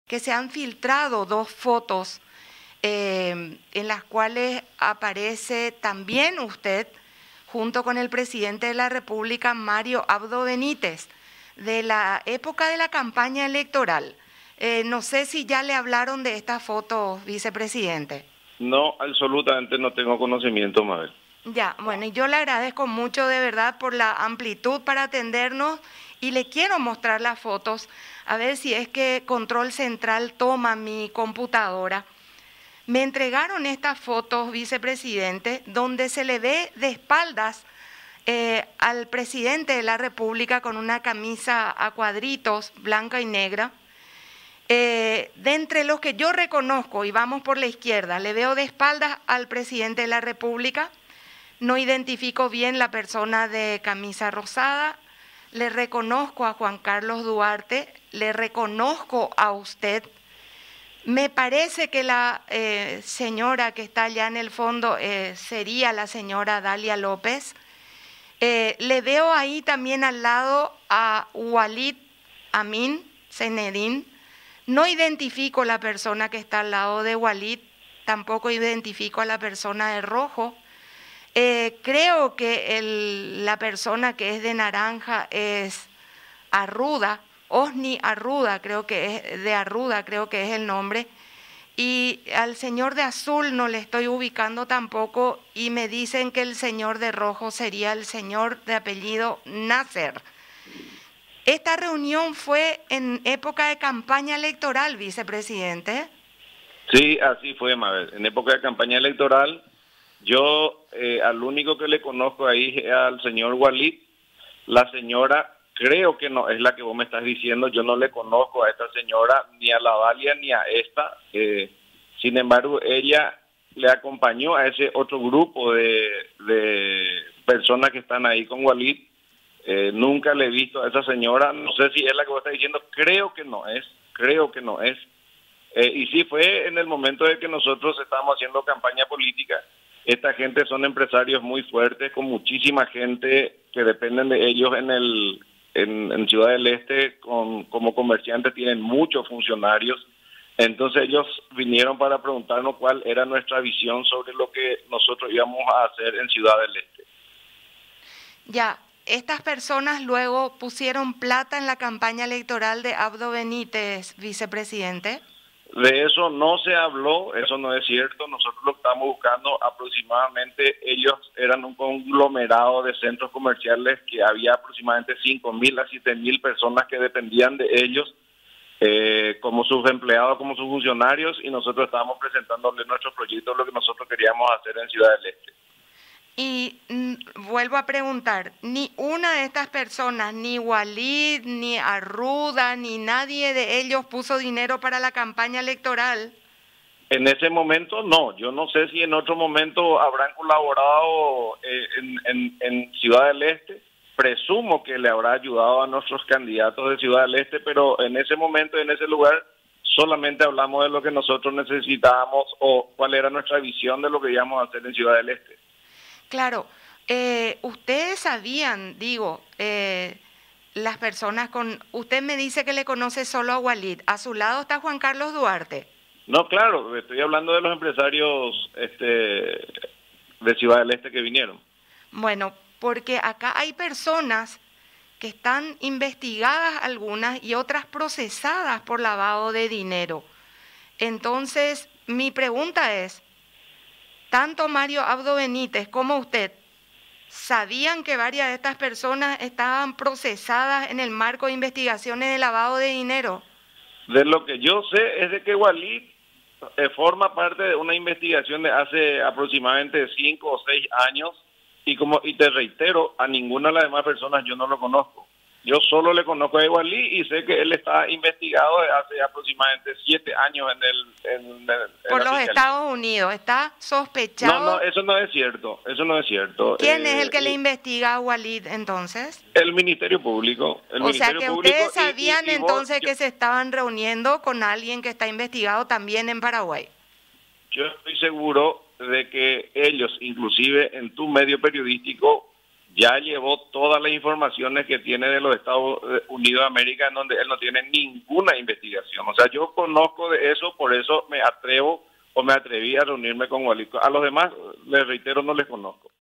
Velázquez desconoce a empresarios con los que Abdo se reunió en campaña
Durante su campaña política, Mario Abdo Benítez se habría reunido con empresarios investigados por presunta evasión impositiva, de acuerdo a imágenes que se dieron a conocer. También participó del encuentro el vicepresidente Hugo Velázquez, quien indicó que la reunión fue a fin de presentar al gremio de comerciantes de Alto Paraná el proyecto gubernamental para esa zona del país.